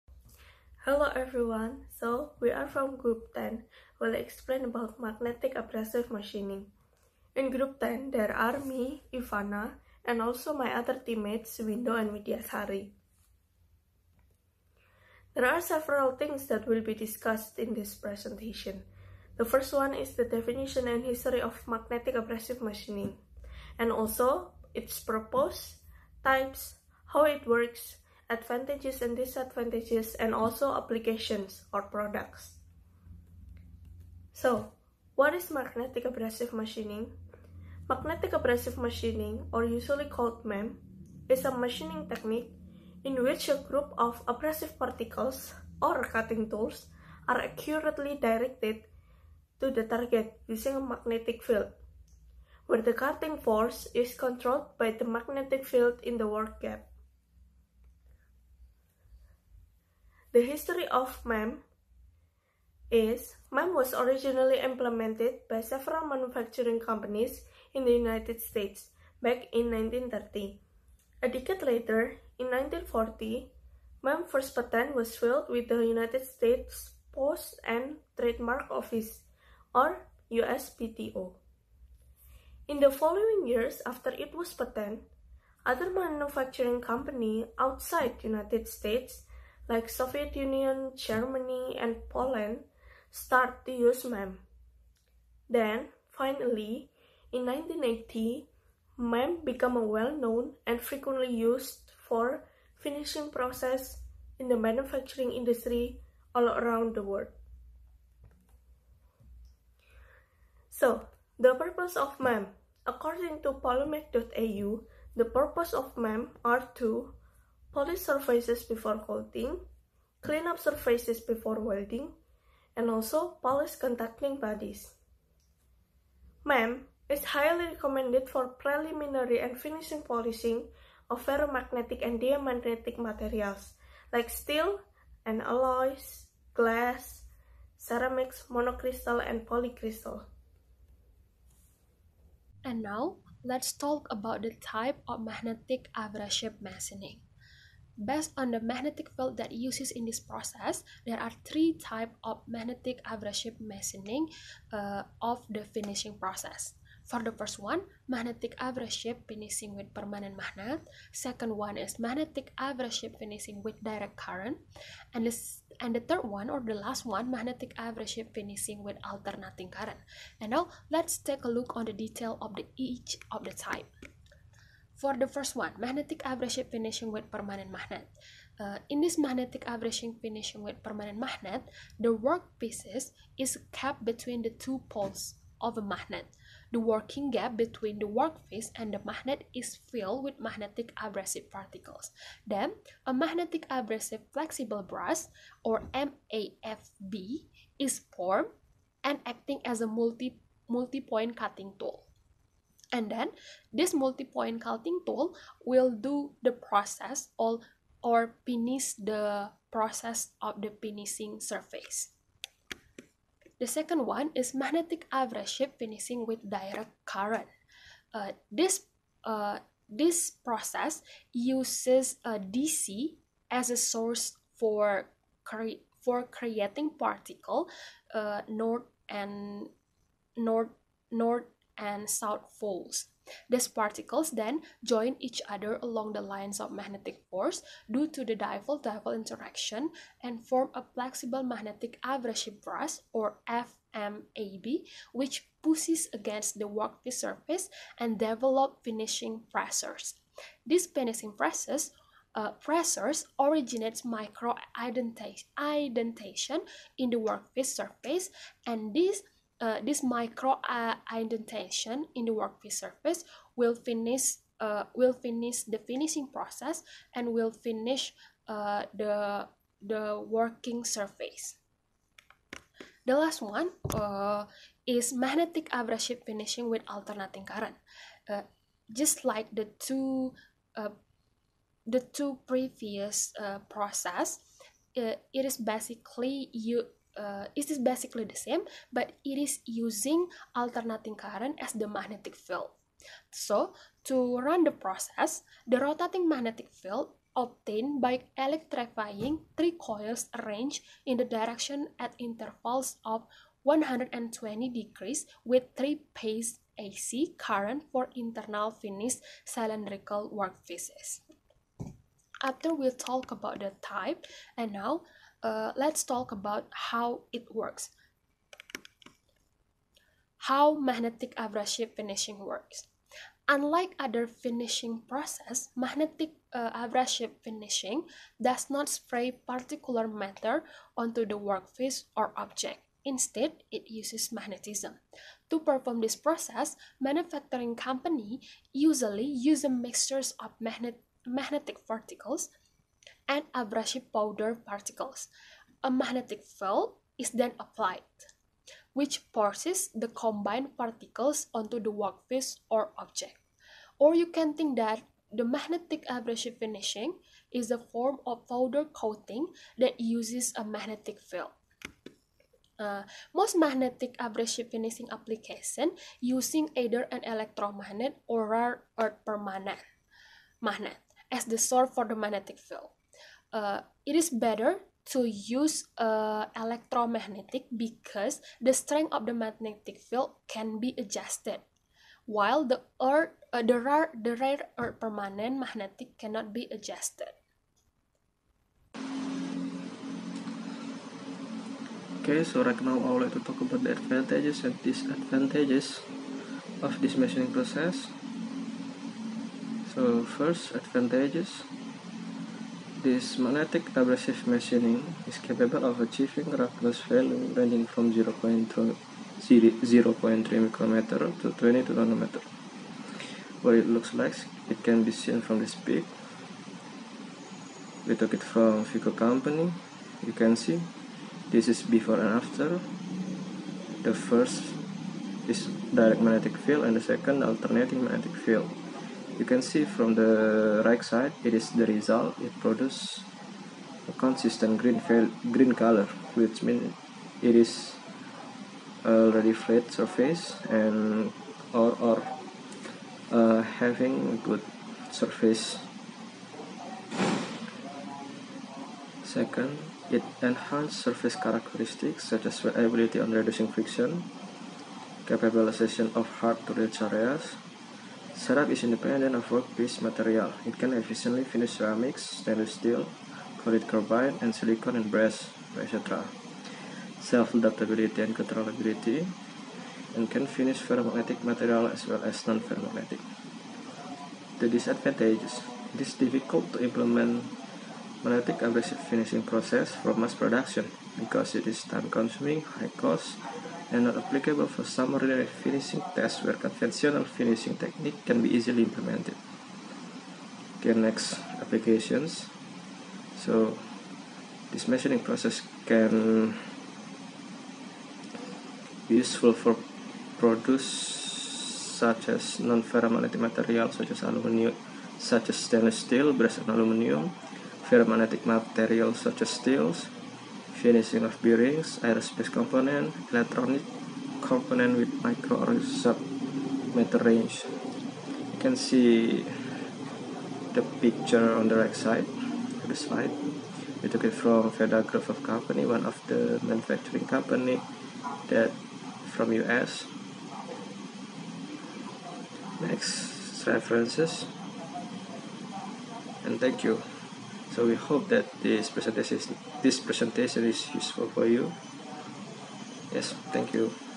Magnetic Abrasive Machining Group 10
This video made by Industrial Engineering students from Sampoerna University in Manufacturing Engineering course Fall semester 2021.